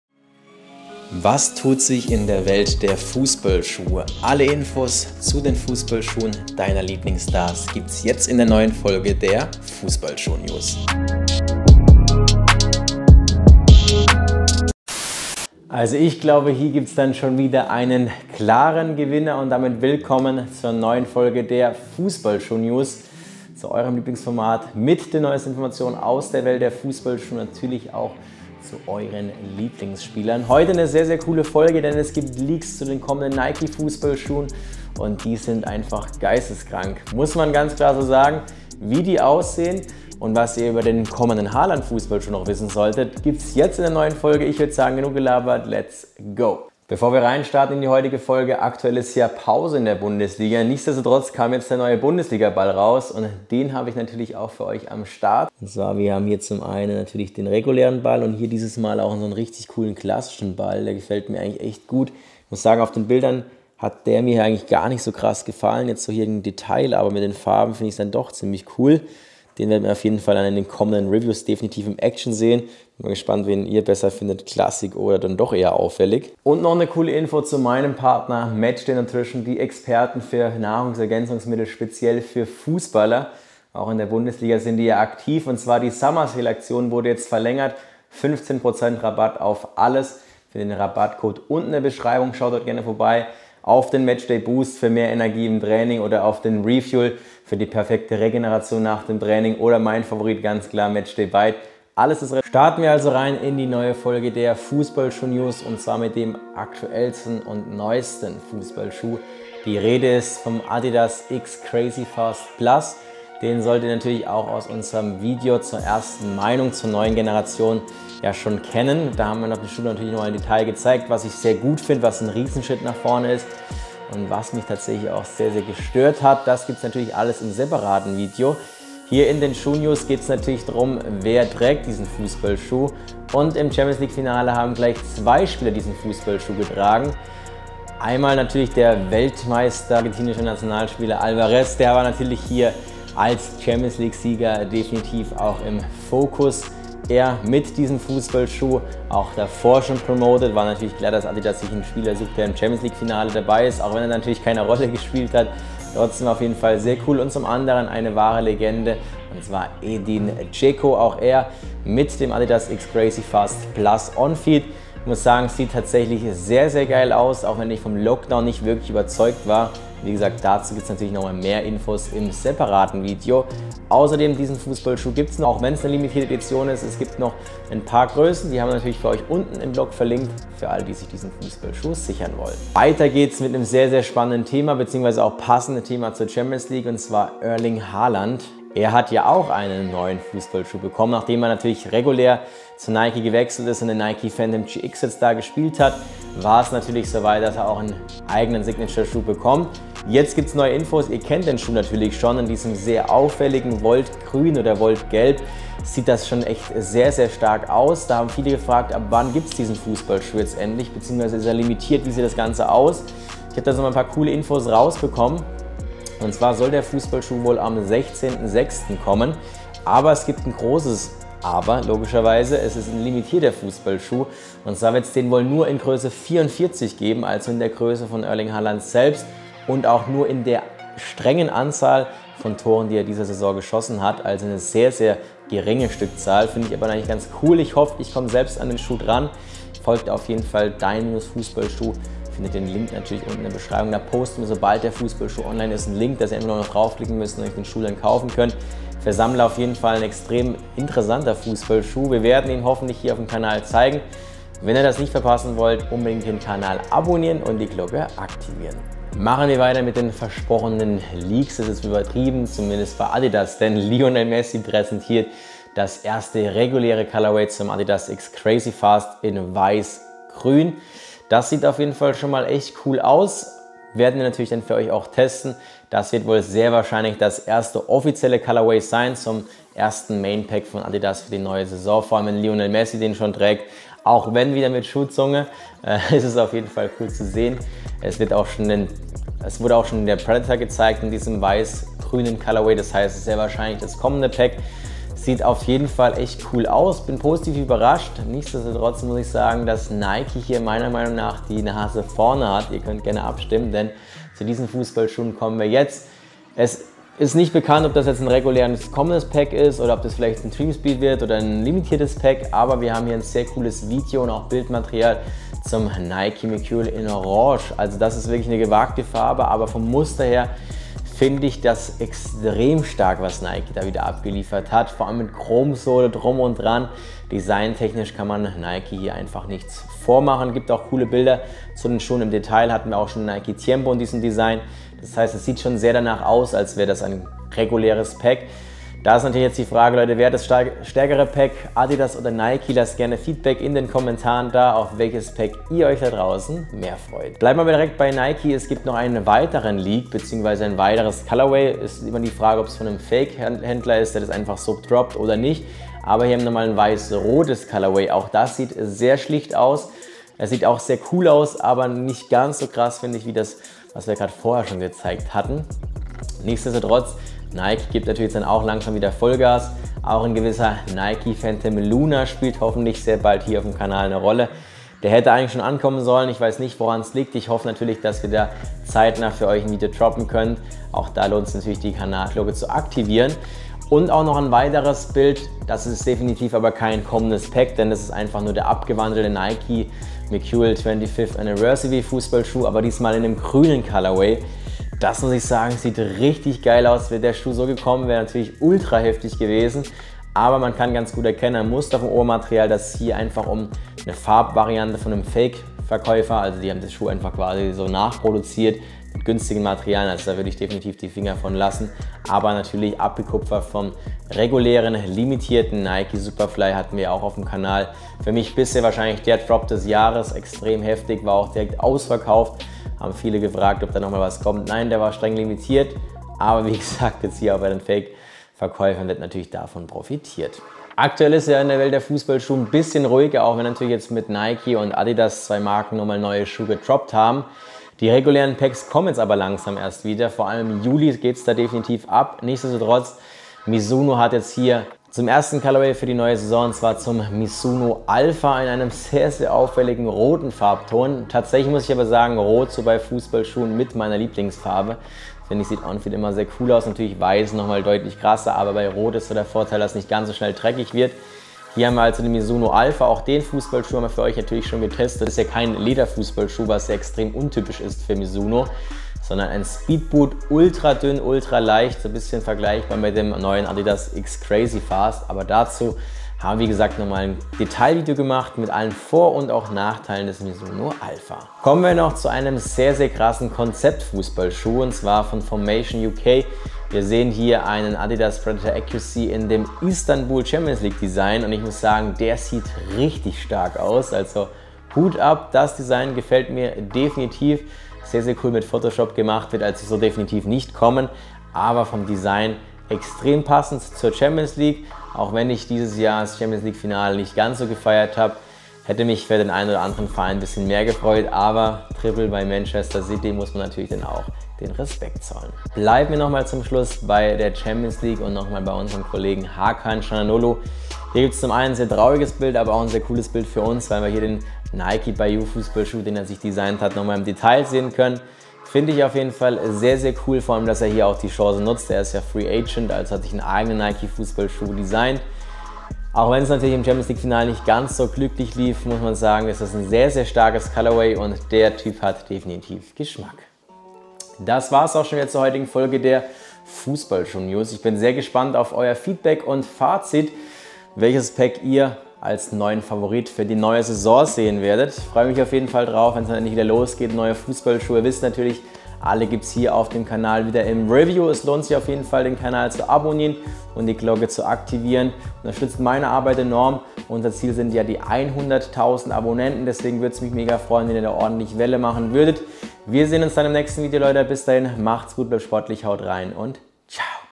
Da geht das Duell halt wieder eindeutig aus...
Da geht das Duell wohl eindeutig aus! Willkommen zu einer neuen Folge Fußballschuh News, hier auf eurem Fußballschuh Kanal Nummer 1 - In der heutigen Video geht es unter anderem um die kommenden Pack von Adidas und Nike und welche Marke hier die Nase vorne hat! Außerdem mit dabei ist der neue X Crazyfast+ und alle Infos zum kommenden Haaland Signature Schuh! Alle Infos zu den Fußballschuhen deiner Lieblingsspieler gibt es jetzt in der neuen Folge Schuh News!